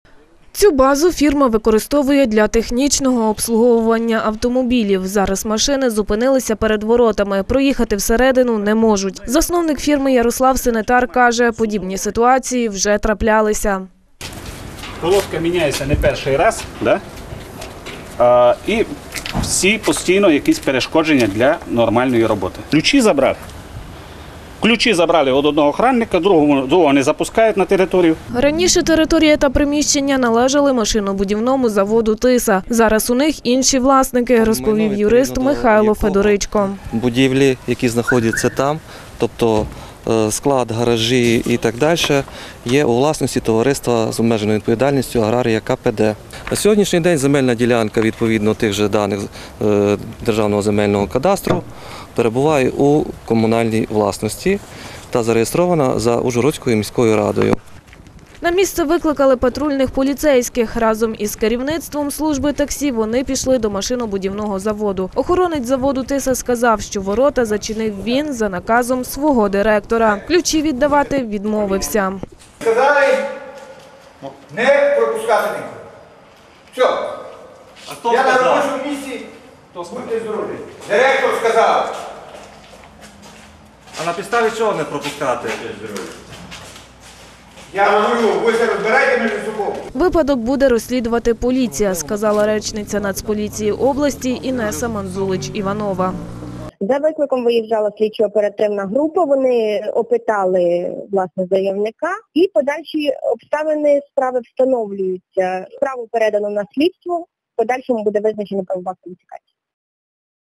Базу обслуговування служби таксі «Лаймджет» заблокували
Базу обслуговування служби таксі «Лаймджет» заблокували. Сталося це вранці 2 листопада. Охоронця, котрий відпрацював свою зміну, випустили із території, того, котрий заступав, - не впустили, розповів засновник фірми Ярослав Синетар. Водії працюють у звичному режимі.